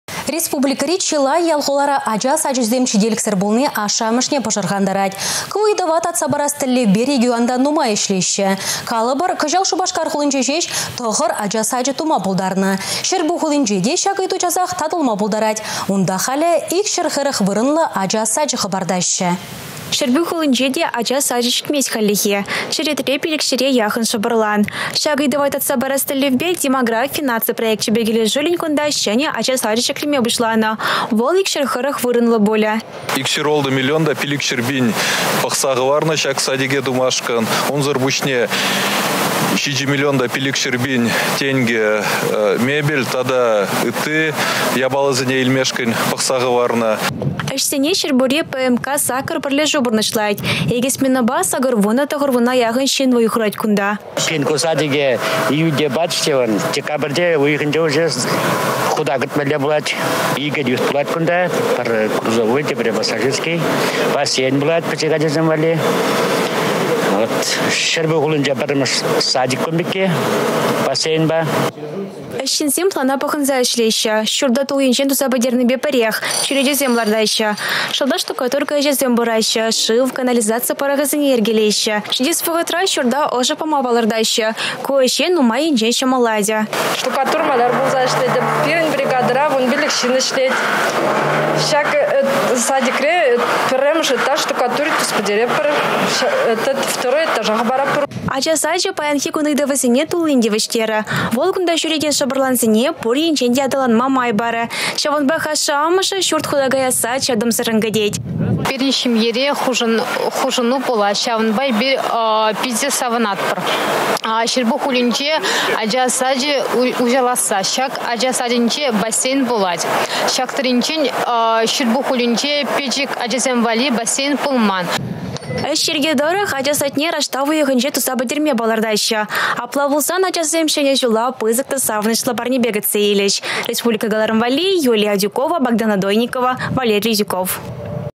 Çěрпӳ хулинче çěнě ача сачě хута кайě
Республикǎри чылай ял-хулара ача сачěсем çителěксěр пулни ашшě-амǎшне пǎшǎрхантарть. Ку ыйтǎва татса парас тěллевпе регионта нумай ěçлеççě. Калǎпǎр, кăçал Шупашкар хулинче çеç 9 ача сачě тума палăртнă. Çěрпӳ хулинче те ку ыйту часах татăлма пултарать. Унта халĕ 240 вырǎнлǎ ача сачě хǎпартаççě.